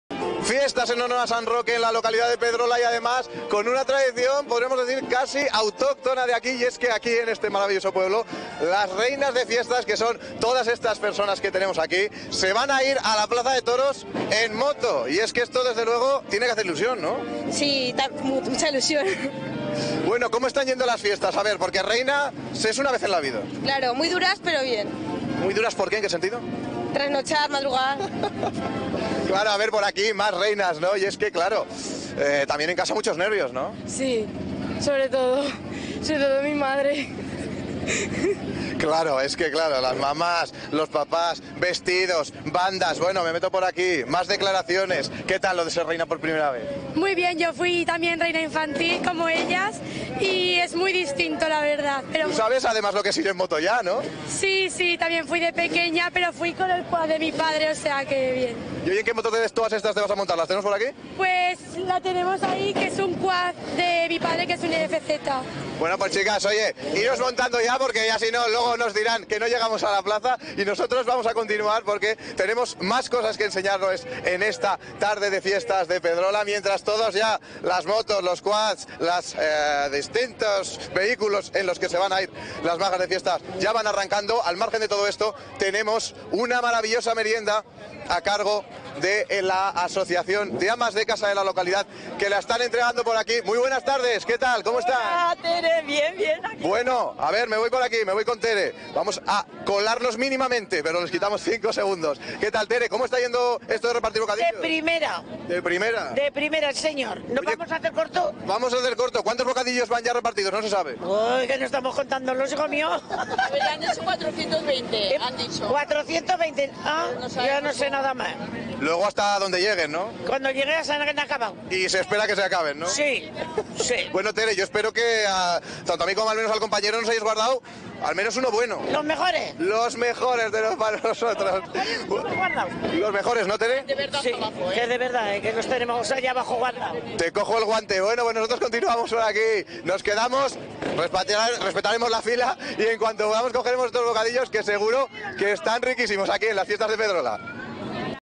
Merienda con moteros y reinas de las fiestas
Los moteros de Pedrola han acercado a las reinas de fiestas hasta la merienda que han preparado las mujeres del pueblo y el ayuntamiento. Allí han podido tomarse unos de los 500 bocatas de jamón que reparten.